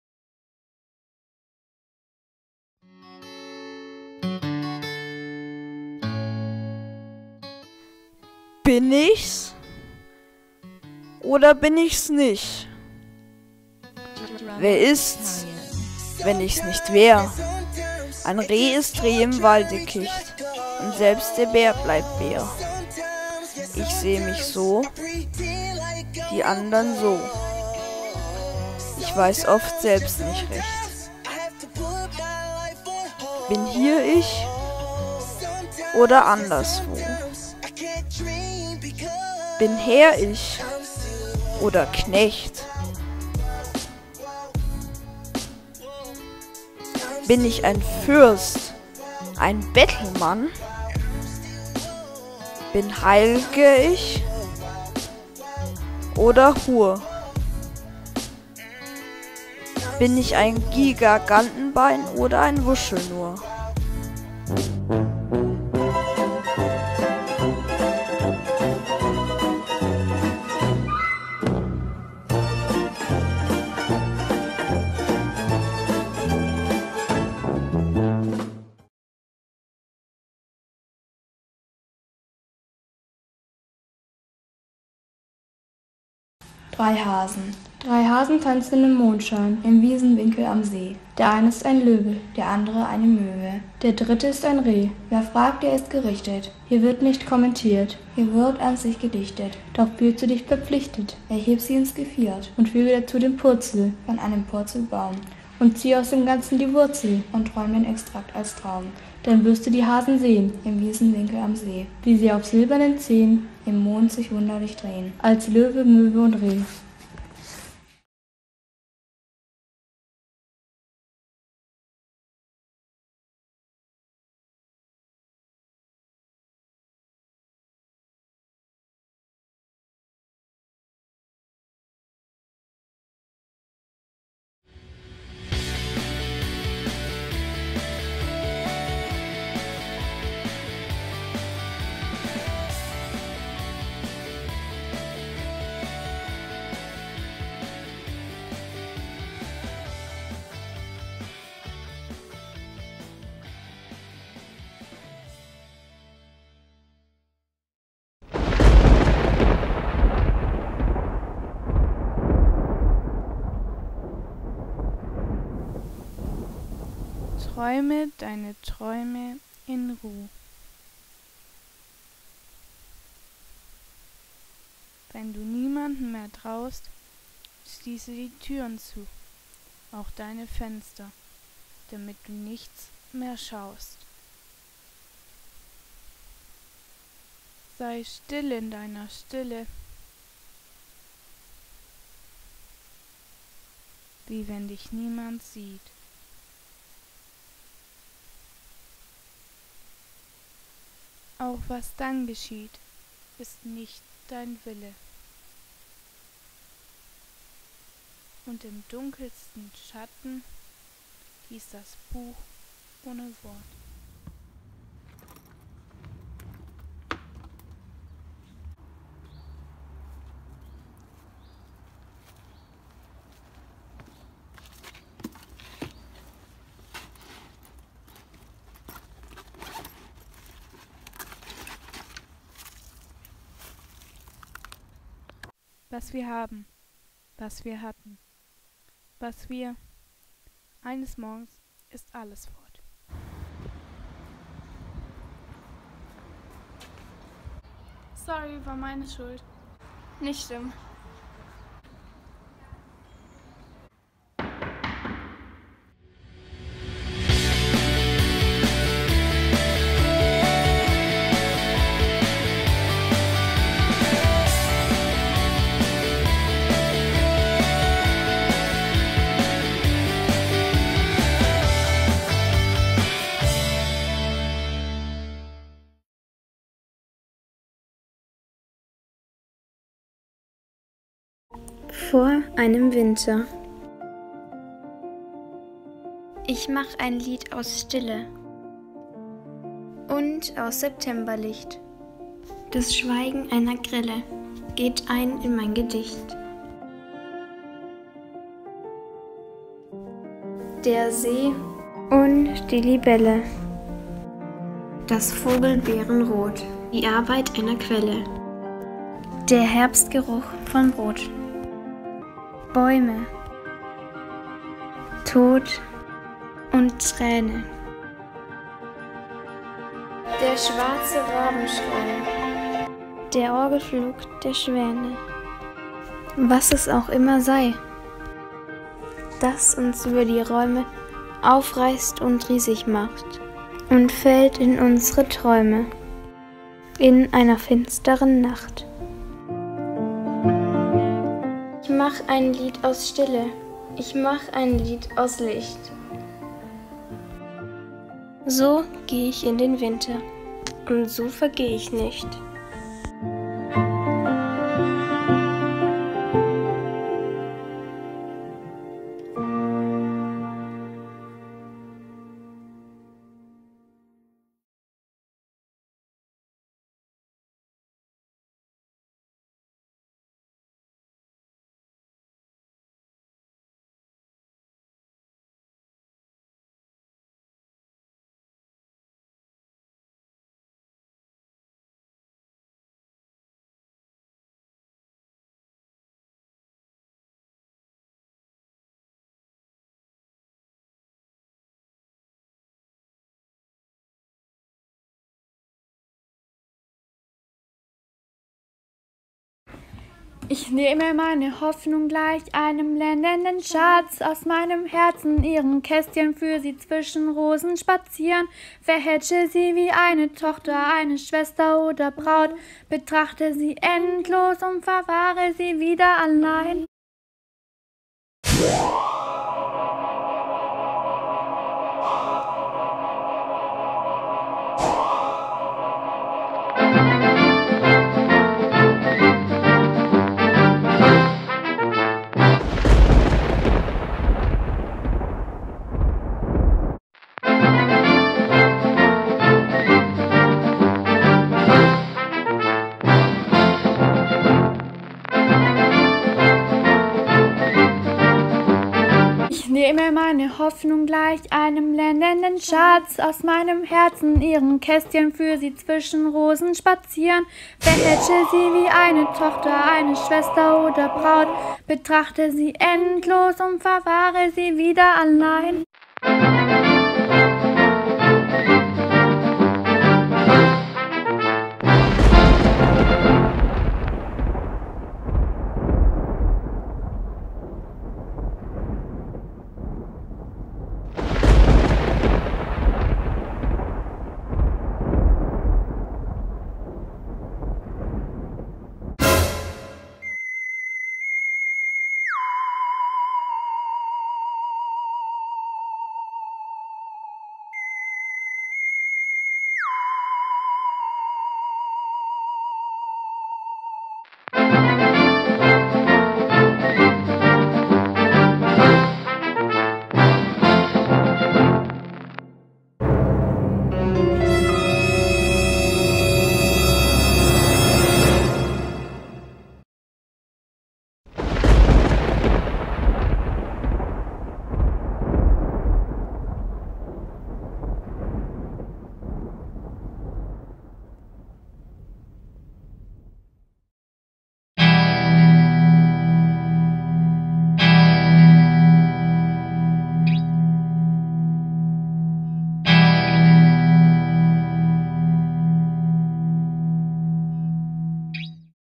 Video Poem Block 2
Der 2. Block aus unserem Projekt Video-Poem:
Lyrik einmal anders  zu vermitteln und den Reiz der digitalen Medien dazu zu nutzen – das war unsere Idee zum Projekt Video-Poem.
SchülerInnen aus drei 8. Klassen produzierten mit Unterstützung der Künstlerin Anna Mönnich, freie Mitarbeiterin der  e.tage, Lyrik-Filme, Poem-Videos, Poetry-Clips … Die Bezeichnungen sind so unterschiedlich, wie es die Ergebnisse sein werden. Ausgangspunkt der Filme ist jeweils ein Gedicht, das zur Grundlage für eine filmische Umsetzung wurde. Emotionen, poetische Gedanken und Aussagen wurden in Filmminiaturen übersetzt und ausgedrückt. Die Buchhandlung Aegis unterstützte das Vorhaben mit Vorschlägen für geeignete Texte und bot damit den SchülerInnen einen Einblick in das Themenfeld Poesie und Lyrik.

Video-Poem ist ein Projekt der e.tage medien.bildung in Kooperation mit dem Kepler-Gymnasium Ulm, der Buchhandlung Aegis Ulm und dem Kino Obscura Ulm.